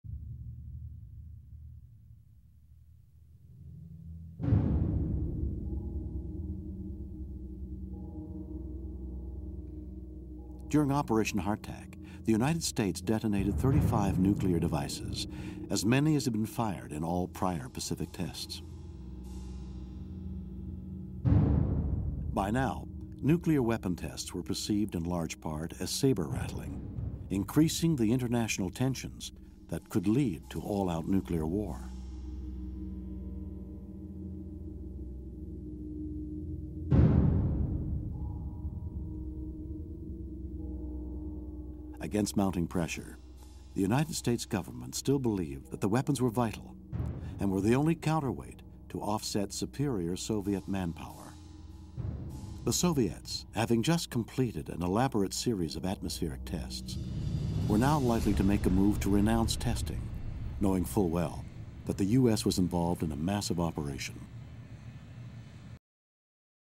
operation hardtack
Operation HARDTACK  consisted of 35 nuclear tests conducted at the Pacific Proving Ground between April 28 and August 18, 1958. These tests included balloon, surface, barge, underwater, and rocket-borne high-altitude tests